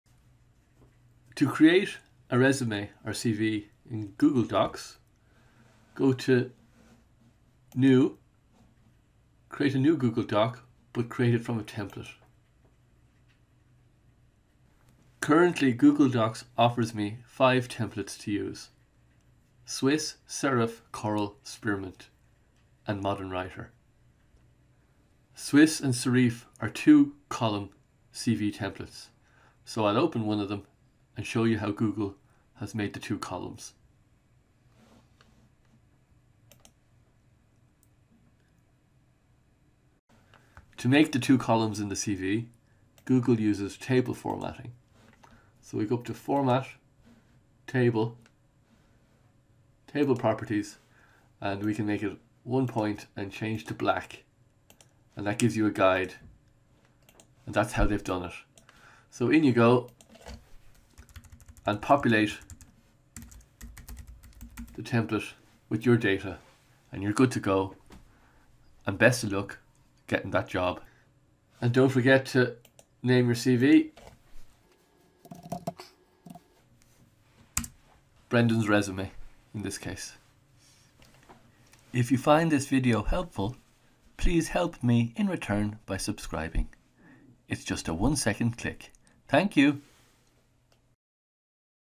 How to Make a Resume on Google Docs in Google Drive
To create a Resume in Google Docs. Open a new Google Doc in your Google Drive, but use the Template option. Google Docs then presents you with a list of templates organized under certain categories. Currently, you will find Google Docs presenting you with 5 Resume templates under their CV Template heading which you can use. Select one CV template and then save it as your document and get to work populating the dummy text with your information. Best of luck with your CV and here's to hoping you land that job.